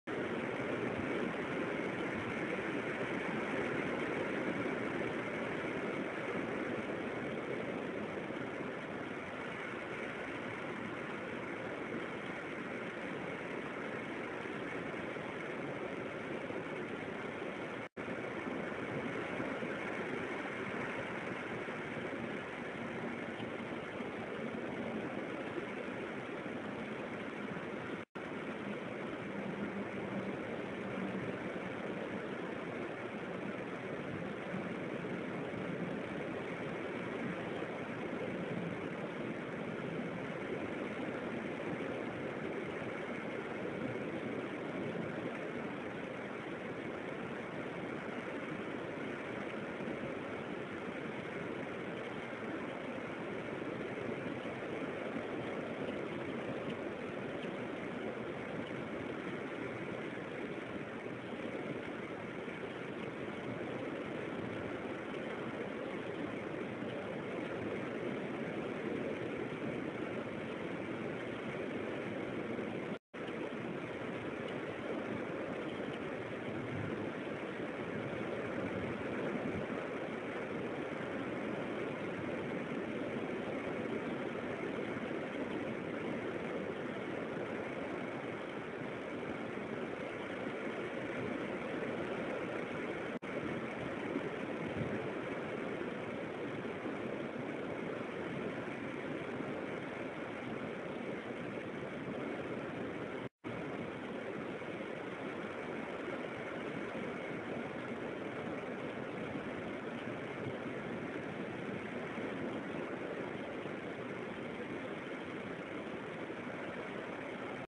3 24 14 Two Harbors early morning, pip watch
K-82 gets up and preens then settles back down.